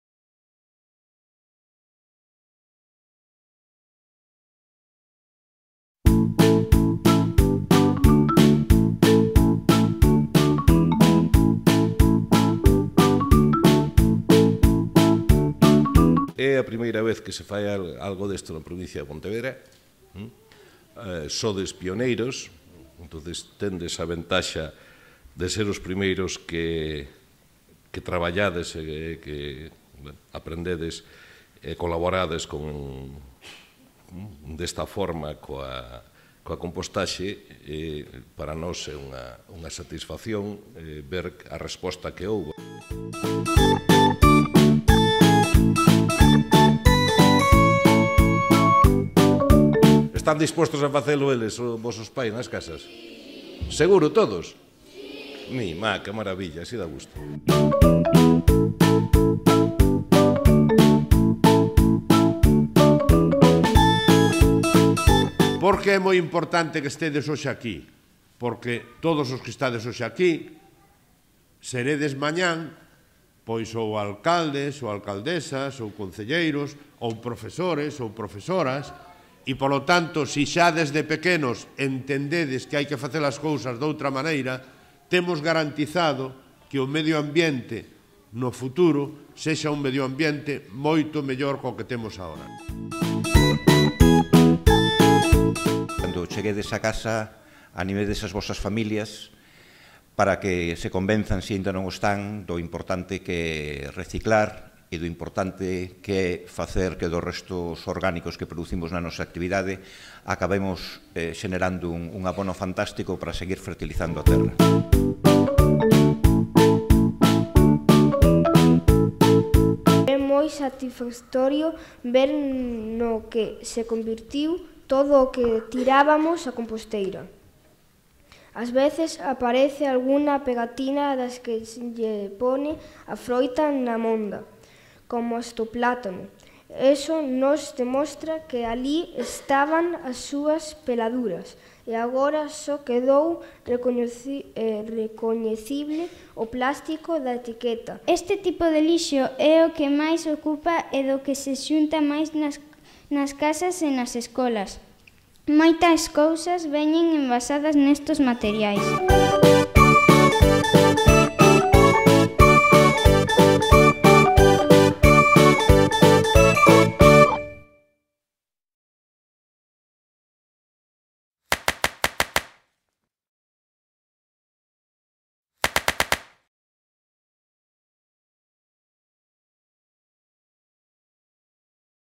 I Congreso de Educación Ambiental sobre Compostaxe para centros de Ensino de Pontevedra, 30 de maio
O I Congreso de Educación Ambiental sobre Compostaxe para centros de Ensino de Pontevedra organizado pola Deputación de Pontevedra ao abeiro do Plan Revitaliza, é inaugurado polo vicepresidente César Mosquera, o director do Centro de Extensión Universitaria e Divulgación Ambiental de Galicia (CEIDA) Carlos Vales e o alcalde Fernández Lores.
Preto de 200 nenas e nenos de seis colexios da provincia de Pontevedra demostran cos seus proxectos escolares que a compostaxe é a forma máis natural de tratar o lixo orgánico.